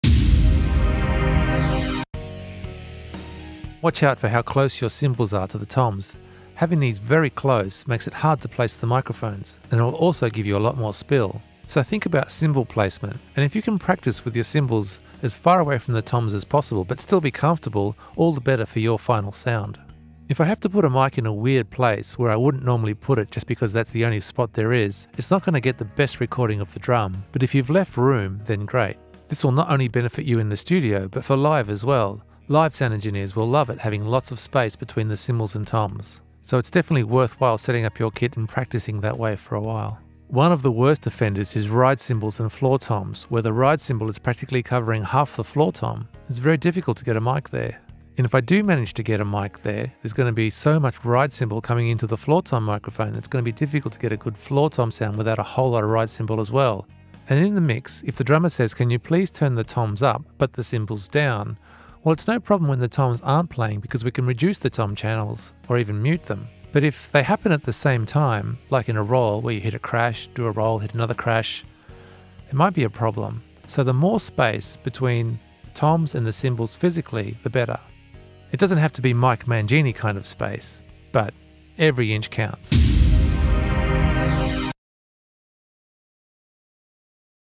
Tips for your first time in the studio - Cymbals and Toms
A web series by Adam Calaitzs to provide information to musicians to help with their first time in a professional recording session

This web series is supported by the
Victorian Government through Creative Victoria.